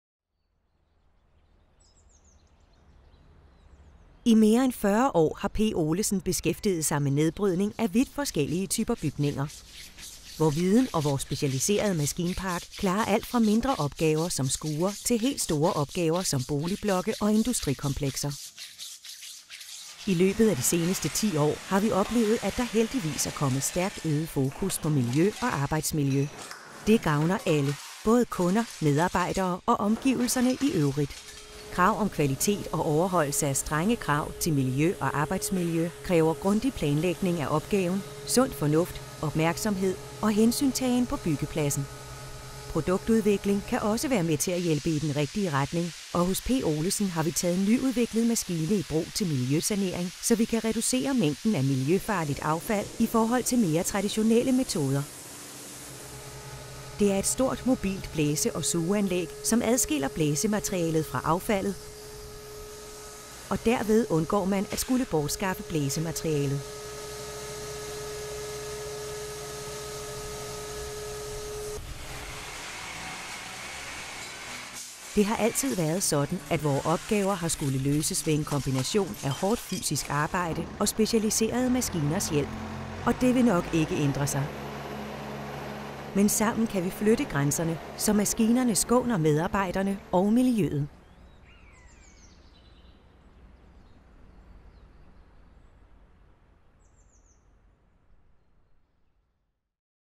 P. Olesen - nedbrydning/miljø
P. Olesen & Sønner A/S er en entreprenørvirksomhed med speciale i nedbrydning, knusning, neddeling af træ samt miljøsaneringer, der indbefatter asbest og miljøgiften PCB.